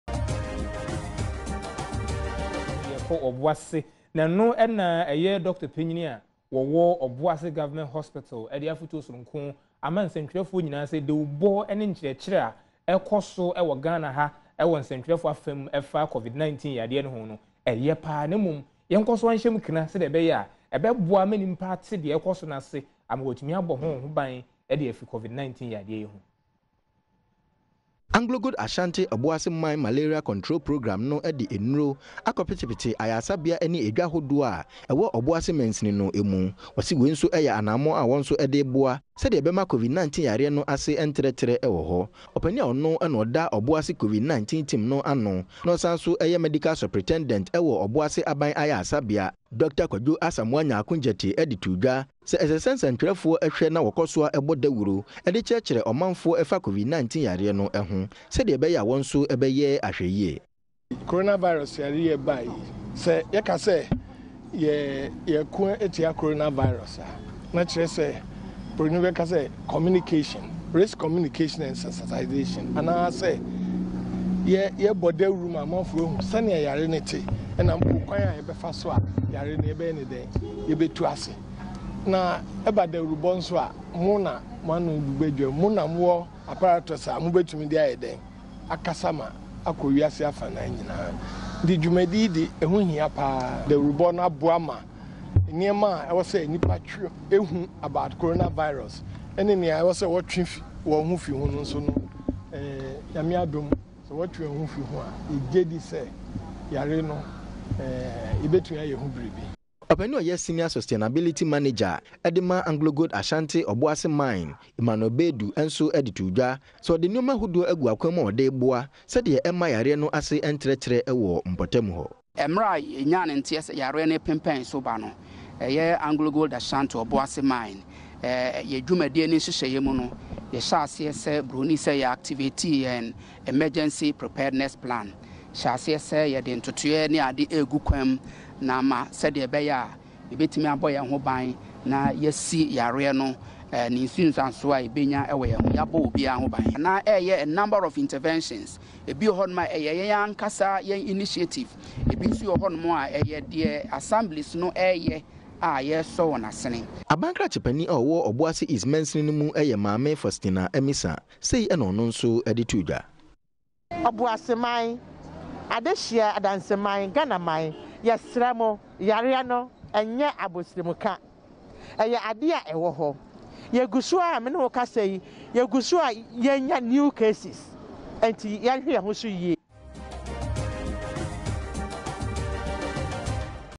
COVID-19 Sensitization - Adom TV News (13-4-20)
Doctor urges media to intensify public education on dos and don'ts